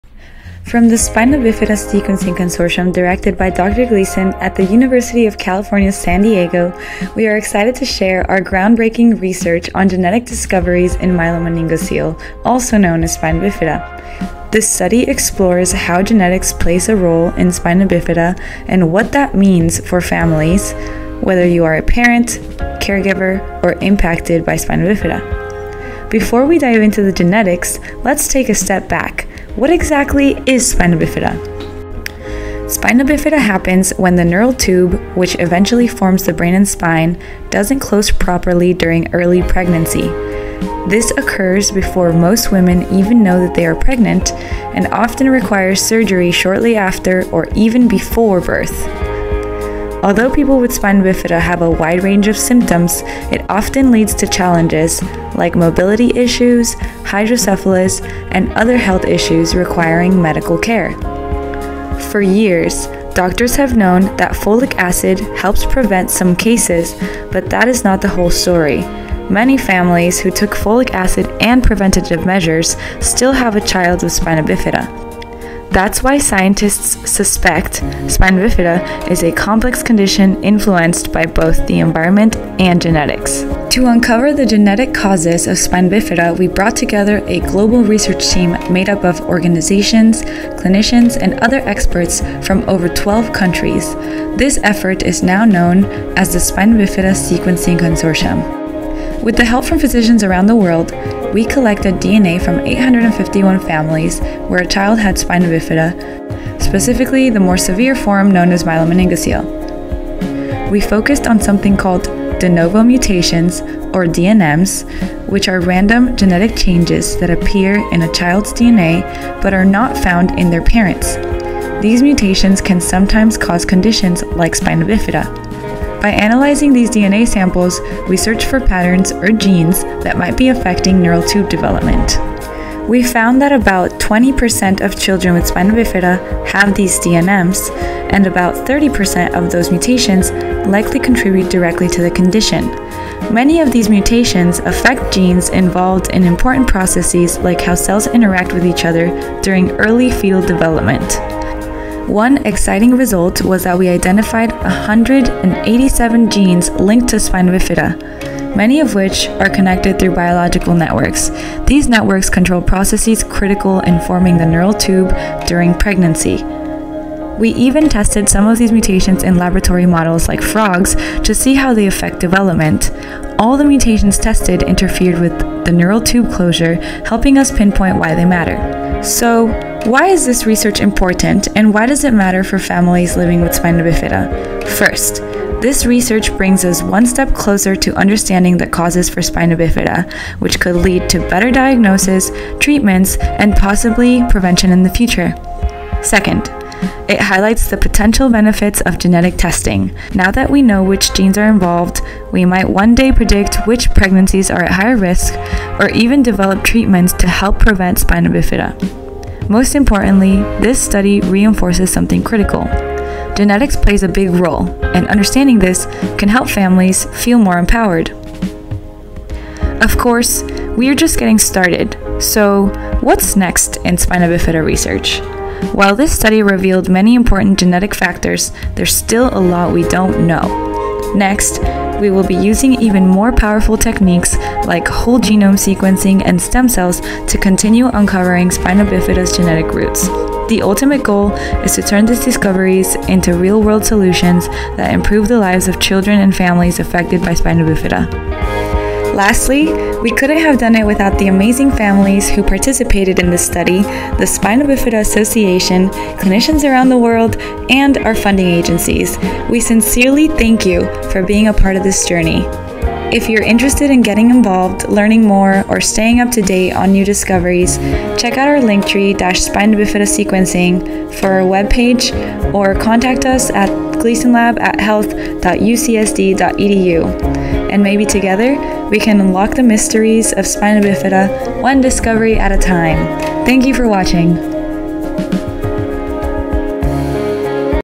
New Genetic Discoveries in Spina Bifida - Gleeson Lab @ UCSD & Spina Bifida Sequencing Consortium
Watch to unlock some of the mysteries of spina bifida research from our latest publication, The Contribution of De Novo Coding Mutations to Meningomyelocele (Ha et al., 2025), published in Nature. Our study is ongoing, and the Spina Bifida Sequencing Consortium continues to grow.

Thank you to:
Participating Families
Spina Bifida Association (SBA)
National Institutes of Health (NIH)
Gabriella Miller Kids First Pediatric Research Program (GMFK)
Spina Bifida Sequencing Consortium members

Video Credits: Naomi Meave Ojeda, Clinical Research Coordinator